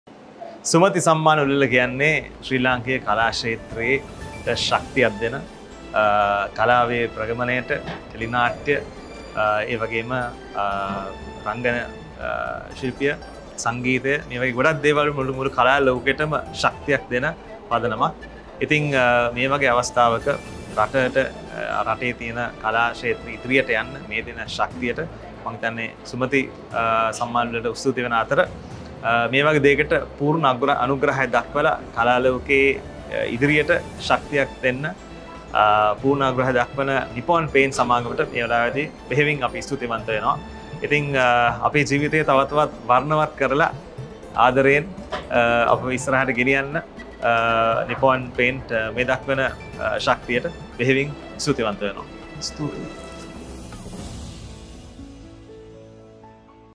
Nippon Paint Lanka was the proud Title Sponsor for the Sumathi Awards 2023!
Mr.Bathiya Jayakody & Mr.Santhush Weeraman extend their gratitude to the Sumathi Awards 2023 for their continued support of artists. A special thank you to Nippon Paint Lanka for being a wonderful Title Sponsor and coloring the lives of these talented stars! This recognition inspires them to create even more music magic.